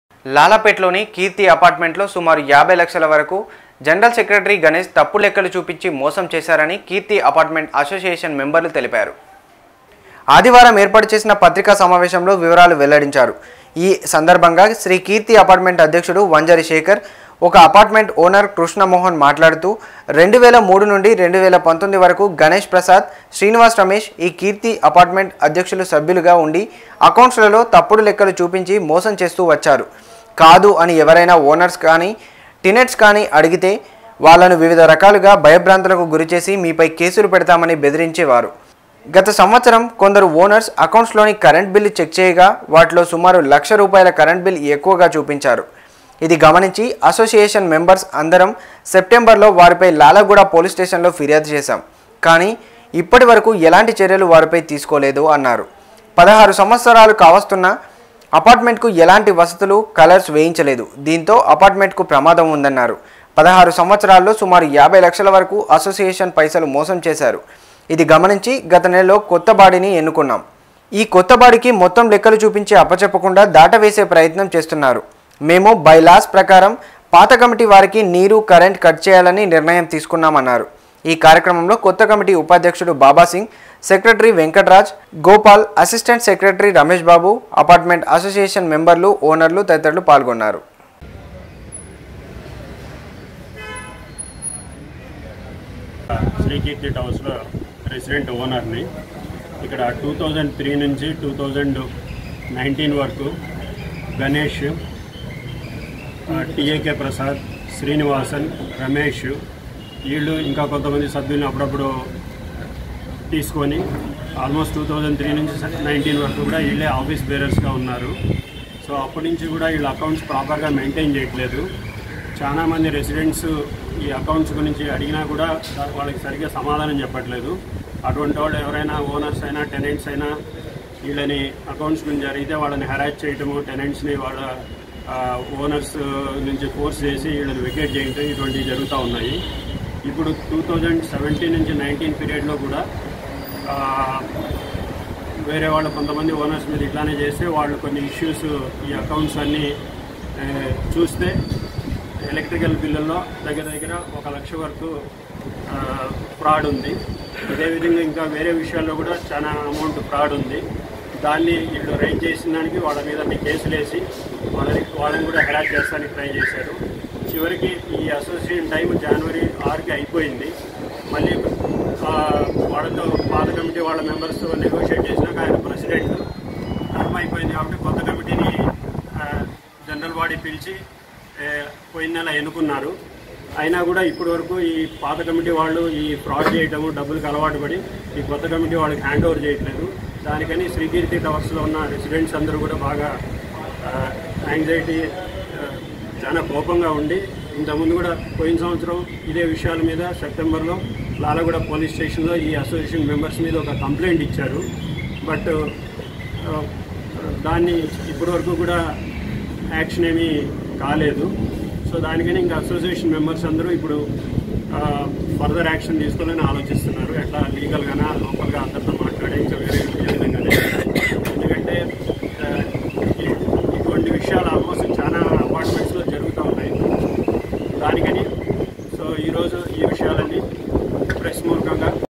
TS24 News తప్పుడు లెక్కలు చూపించి మోసం చేశారు అంటున్న శ్రీ కీర్తి అపార్ట్మెంట్ మెంబర్లు
లెక్కలు చూపించి మోసం చేశారు అంటున్న శ్రీ కీర్తి అపార్ట్మెంట్ మెంబర్లు